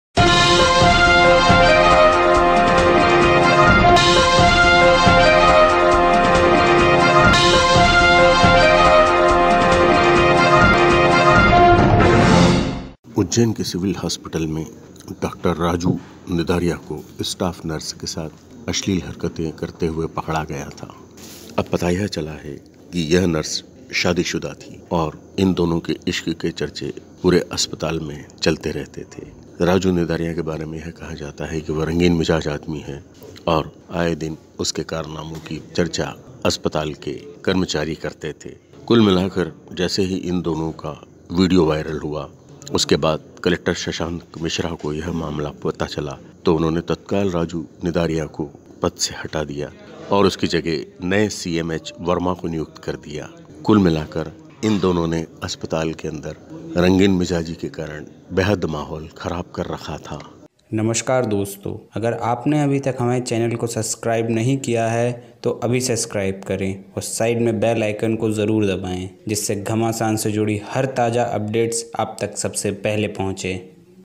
Ujjain | Civil Surgeon शादीशुदा Nurse और Doctor के कारनामे की थी चर्चा अस्पताल में
Civil Surgeon and Nurse... Kissing Video inside Operation Theatre - Ujjain Hospital Goes Viral - वीडियो वायरल होने के बाद कलेक्टर ने हटाया पद से